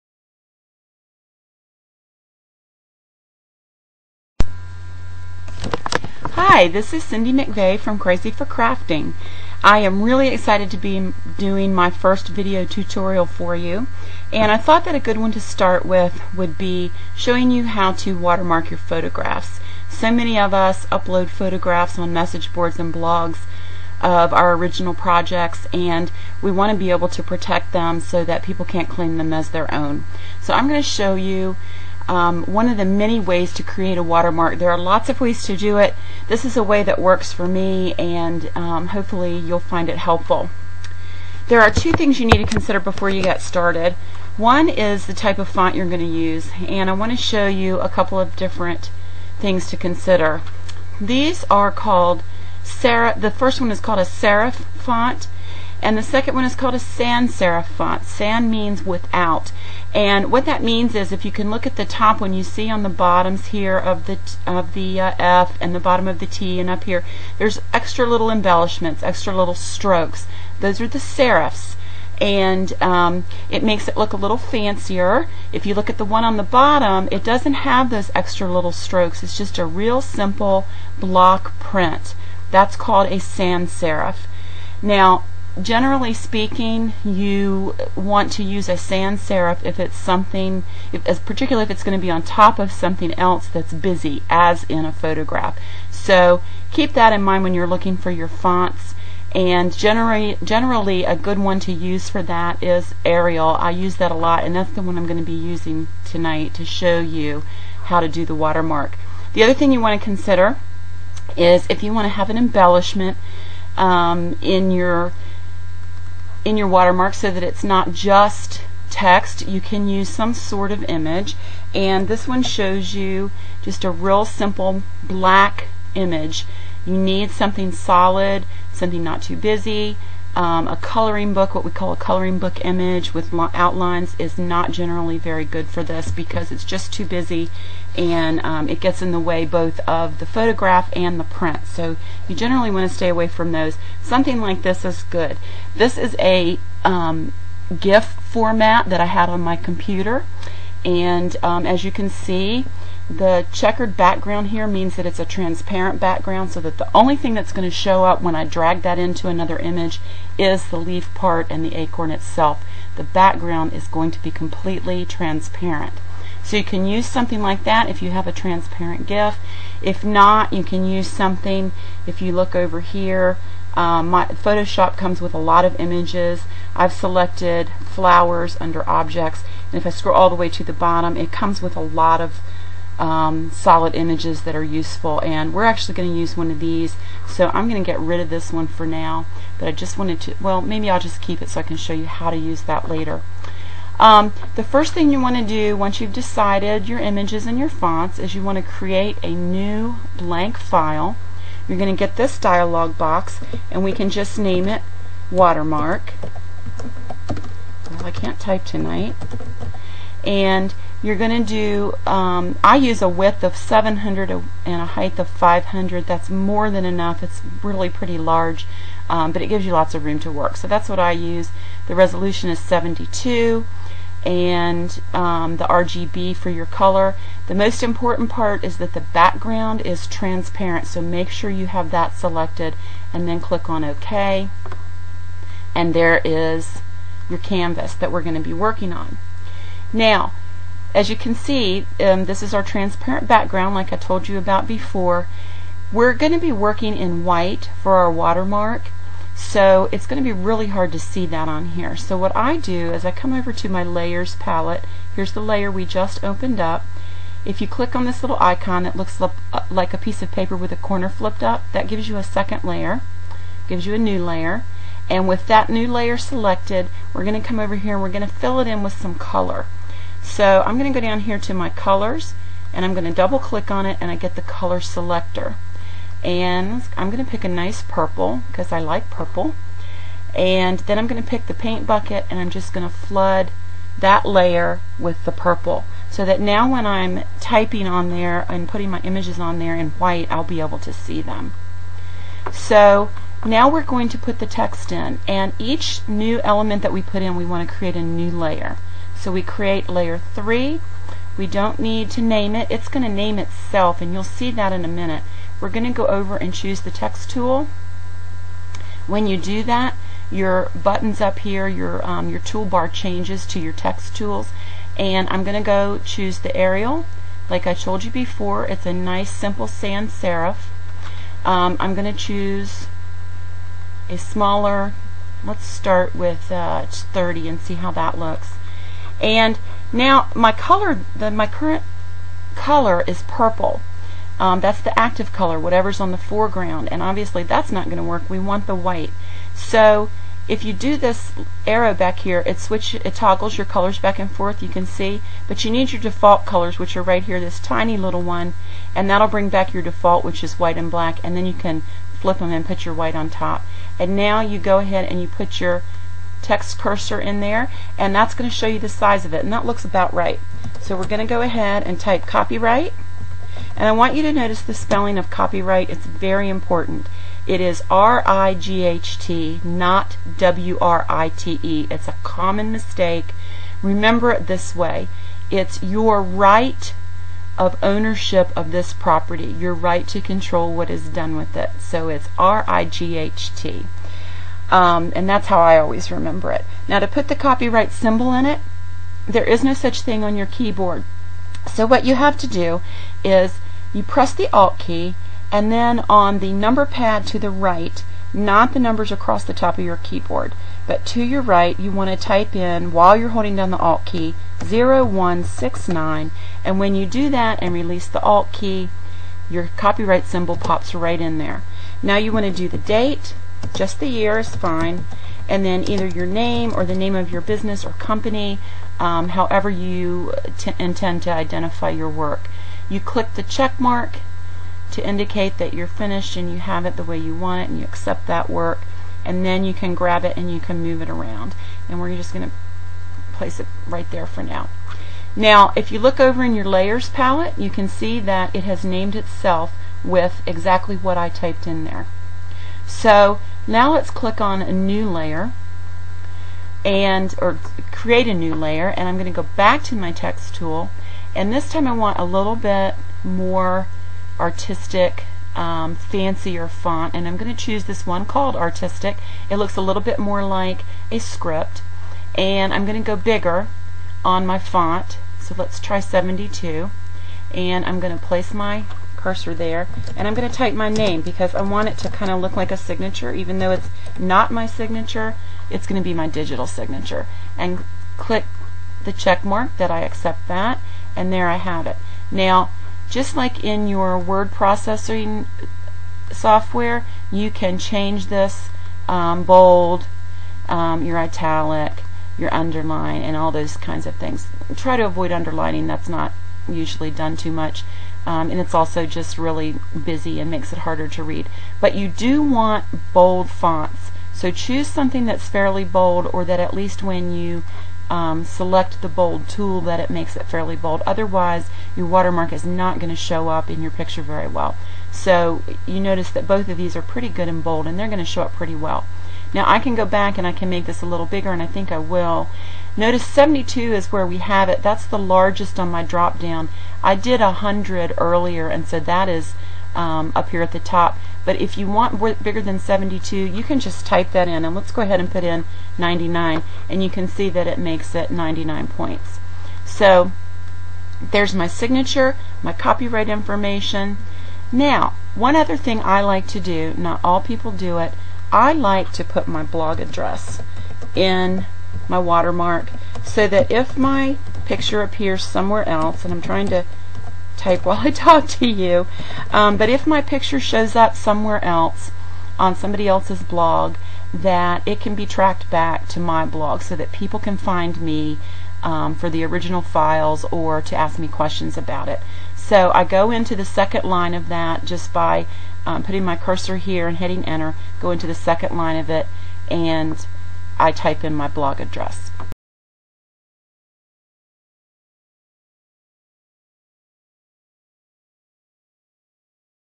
Creating and Using a Watermark - Tutorial, Part 1
for more tutorials and Cricut craft projects.  Learn to create and use a multi-part decorative watermark in your images, including copyright information. Uses Photoshop Elements.